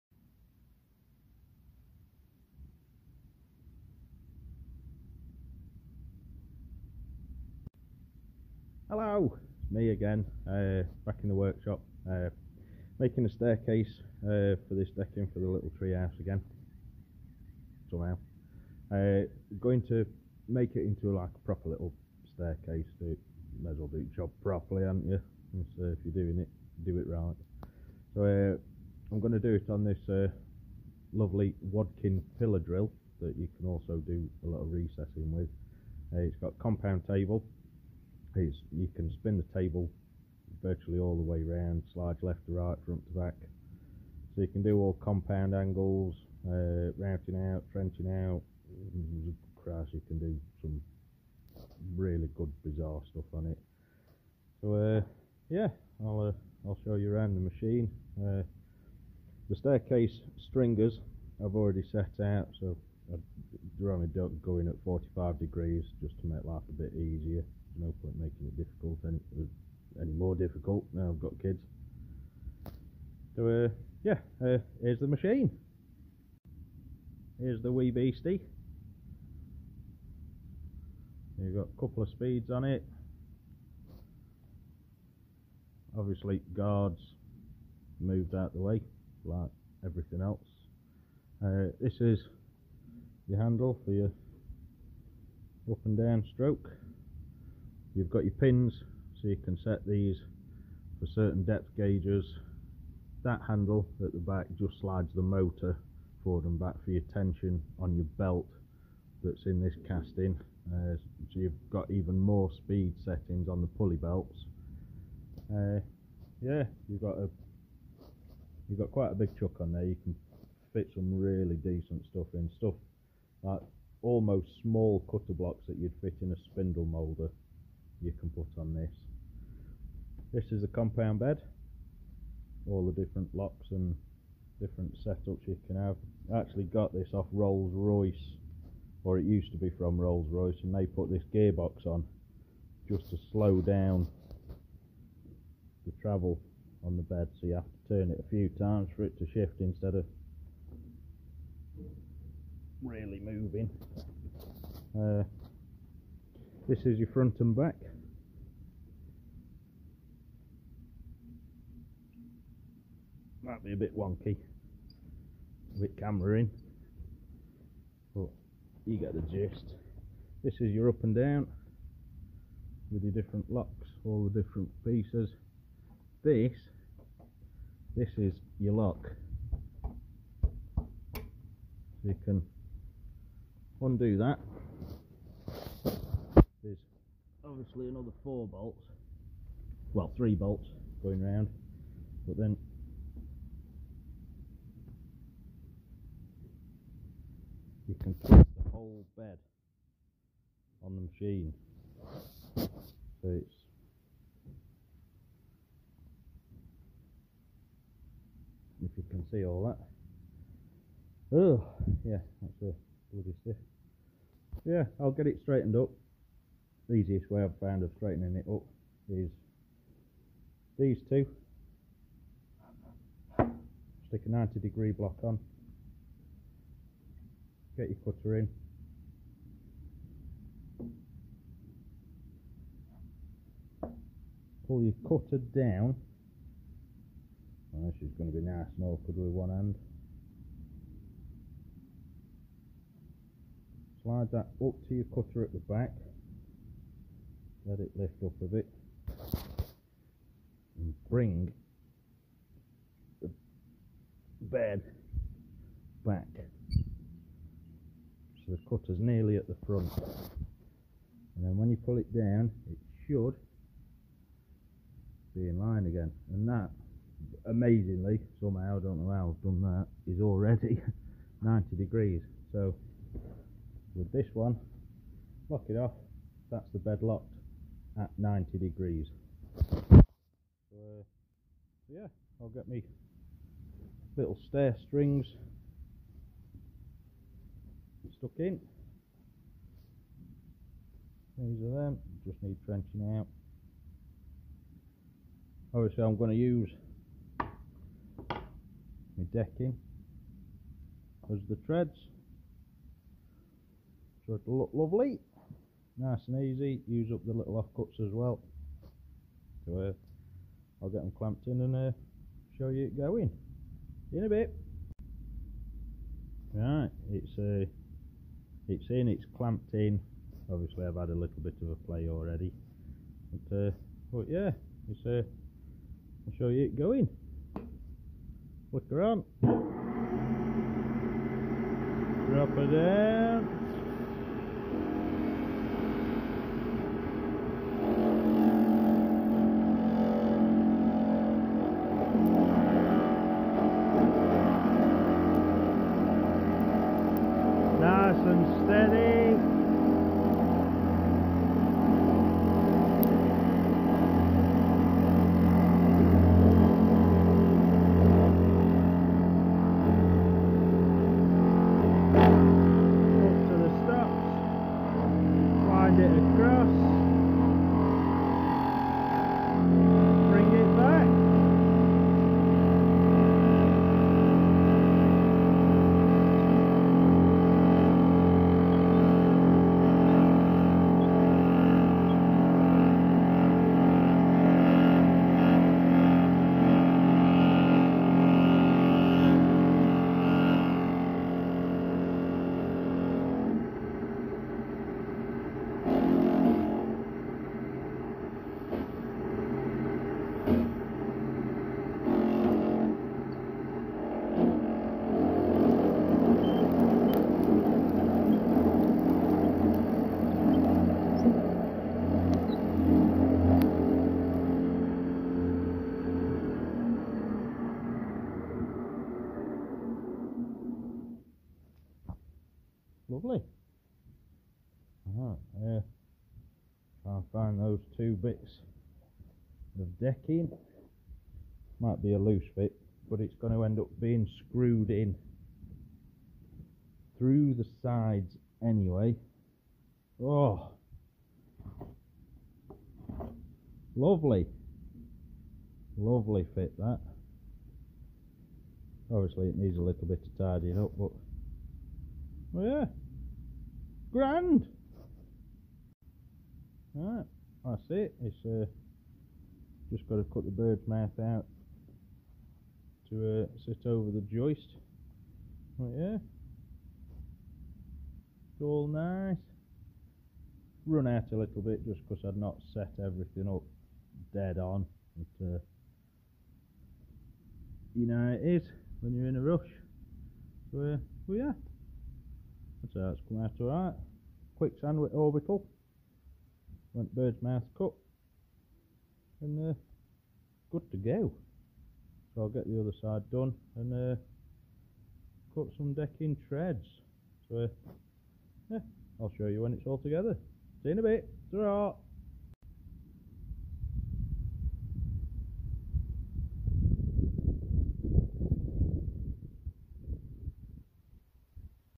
How to make a staircase, for the little tree house.
Not sure what's happening with the hair but the wadkin still does a bloody good job.